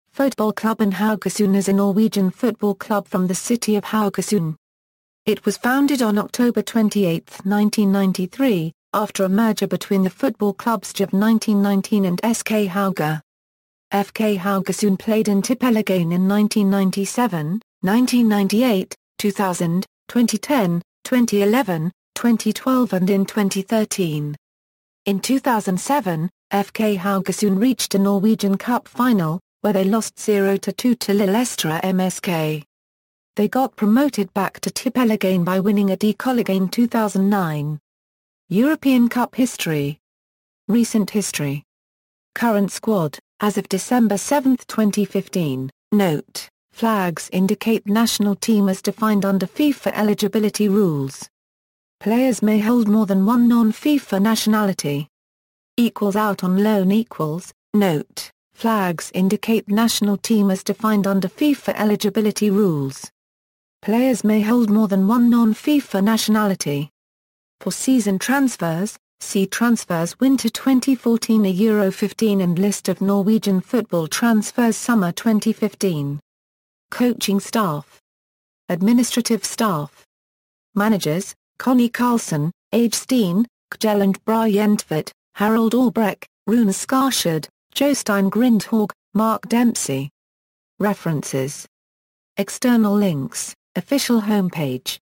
FK Haugesund
Fotballklubben Haugesund is a Norwegian football club from the city of Haugesund. It was founded on October 28, 1993, after a merger between the football clubs Djerv 1919 and SK Haugar. FK Haugesund played in Tippeligaen in 1997, 1998, 2000, 2010, 2011, 2012 and in 2013. In 2007, FK Haugesund reached the Norwegian cup final, where they lost 0-2 to Lillestrøm SK. They got promoted back to Tippeligaen by winning Adeccoligaen 2009.